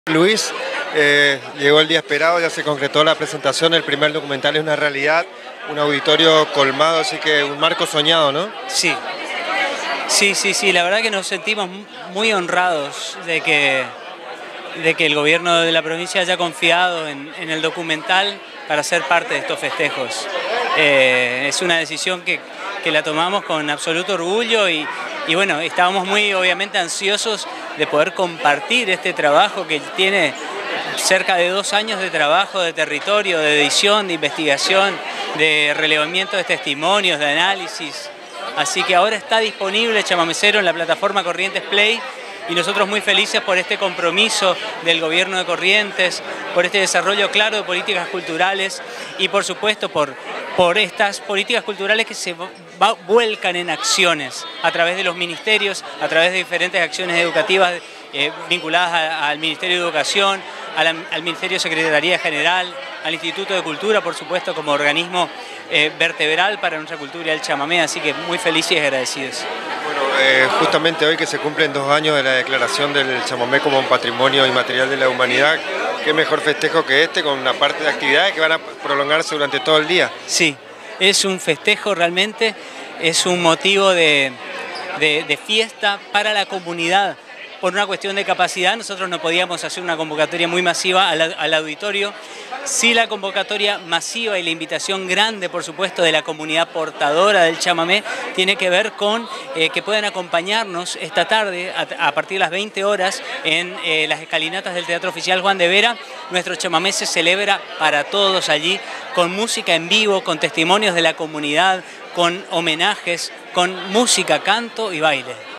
SE PRESENTÓ EL DOCUMENTAL "CHAMAMECERO, NUESTRA MANERA DE SER" EN EL AUDITORIO JULIÁN ZINI
Se trata de la primera serie documental que recorre la historia socio-cultural del fenómeno chamamecero. Cómo surge, vive y se expresa este universo cultural es el propósito del trabajo audiovisual que cuenta con 10 capítulos que se irán estrenando en la plataforma Corrientes Play desde este viernes 16 de diciembre.
Cabe recordar que un 16 de diciembre del año 2020, la Organización de las Naciones Unidas para la Educación, la Ciencia y la Cultura (Unesco) brindaba al Chamamé su mayor distinción por su trascendental aporte a la cultura en todo el continente.
El primero de los diez episodios de 21 minutos de la serie documental fue presentado ante las autoridades provinciales, gestores culturales, músicos y público en general en el auditorio Julian Zini del Centro Administrativo en Corrientes. El trabajo audiovisual realizado por Luis Moulin, Cayo Fernández, Facundo Vallejos Yunes y la participación especial de Julio Cáceres, recupera los rasgos, aspectos y elementos que persistieron por más de 400 años en la memoria y en las prácticas culturales de nuestra gente.
CÁMARA-EDICIÓN: MARCOS ALARCÓN